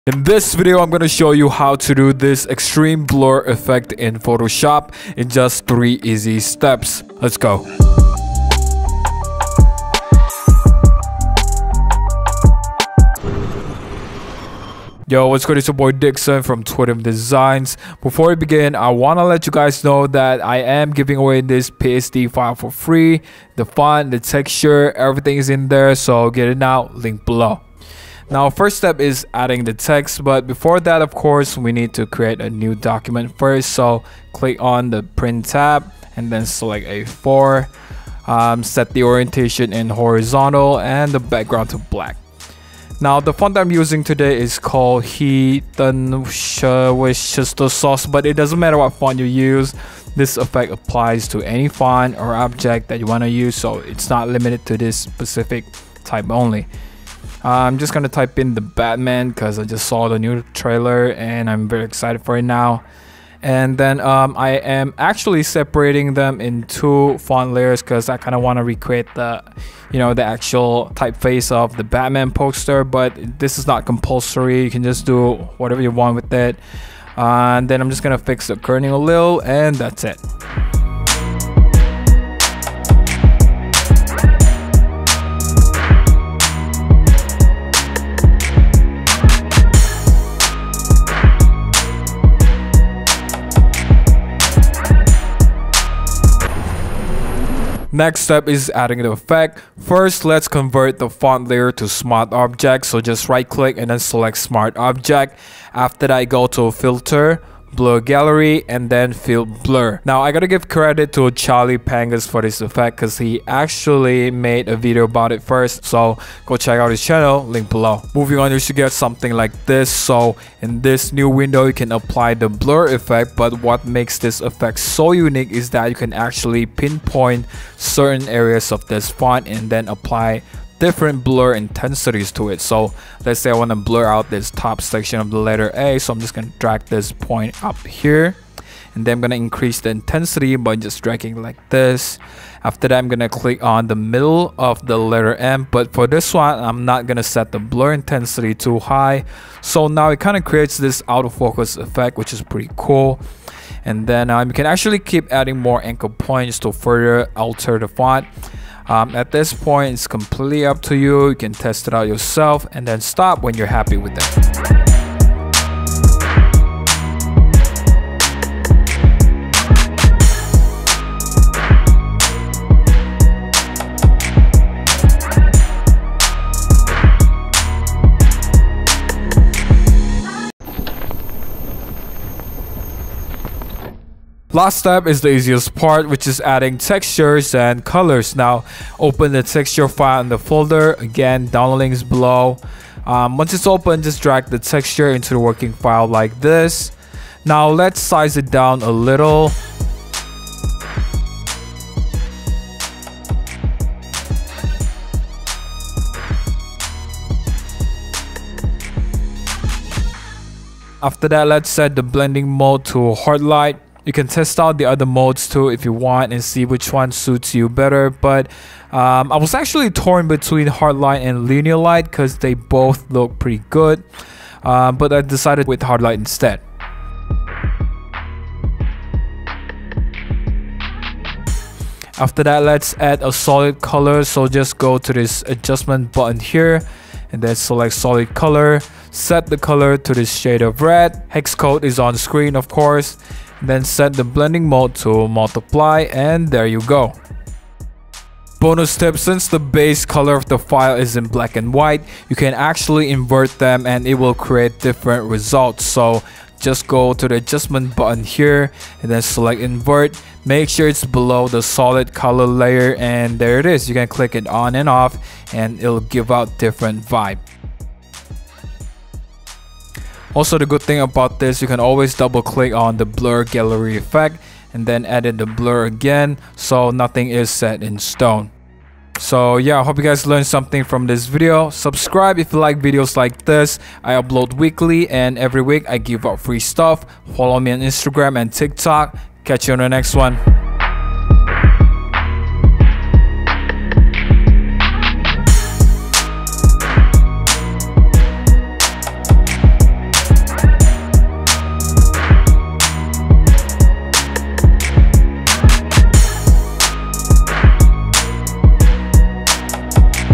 Master This EXTREME Blur Effect In 3 Easy Steps (PHOTOSHOP TUTORIAL + FREE PSD)
Quick and easy photoshop tutorial to master the field blur effect. A convenient technique to learn that can be utilized on many projects. 🤜🤛

TIMESTAMPS:
0:00 Intro
0:12 Step 1
1:36 Step 2
3:00 Step 3